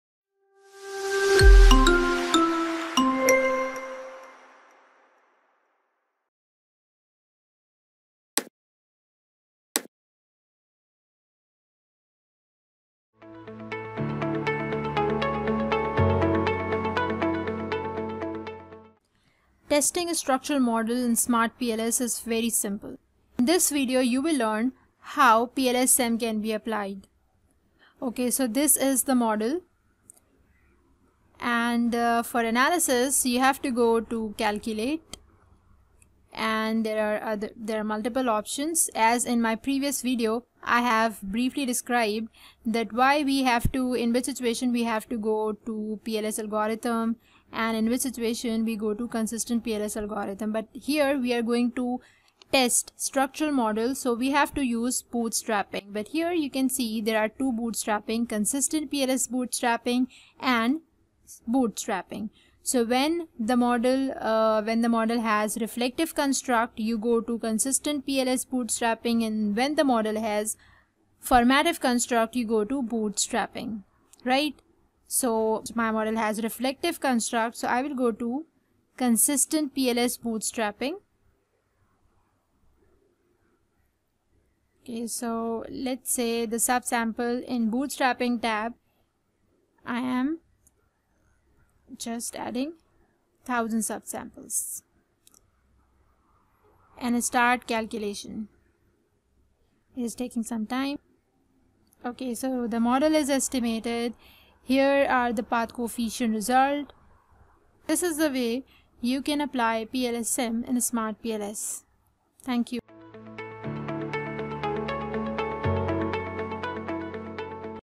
Structural Model (PLS-SEM) in SmartPLS
In this video I will discuss how structural model can be tested in SmartPLS. We will be using PLS-SEM technique.

PLS-SEM in SmartPLS
Structural Model in SmartPLS
QRSchool | Quantitative Research Methods
AND IF THIS VIDEO IS HELPFUL